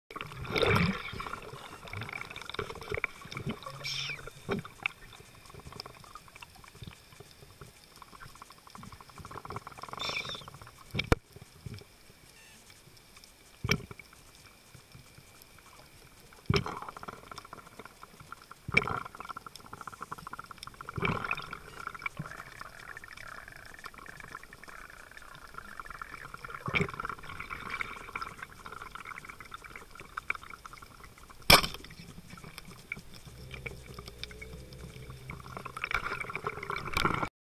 Hogfish Invasion - MOVE!!! I need to shoot that nice Mutton Snapper.
I had hogfish getting my way while trying to get a nice Mutton Snapper today while freediving. I went back and got the hogfish of course because they didn't move. We need to do something about this hogfish issue. Are they an invasive species?